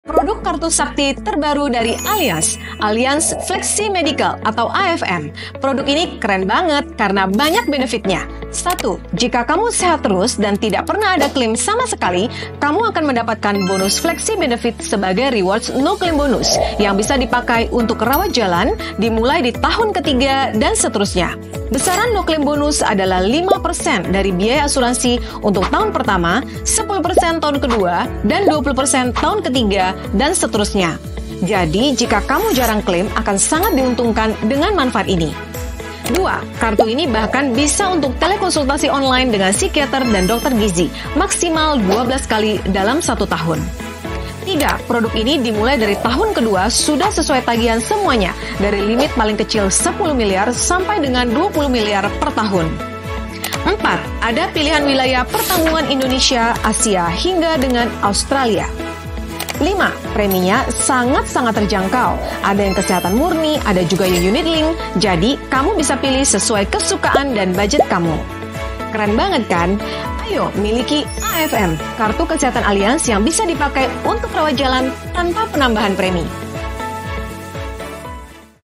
Allianz Flexi Medical (Animation version) | Asuransi Kesehatan Rawat jalan tanpa Tambahan Premi
Terbaruuu.. Asuransi Kesehatan yang bisa dipakai untuk Rawat jalan tanpa tambahan premi. Namanya AFM. Simak langsung yuuuukk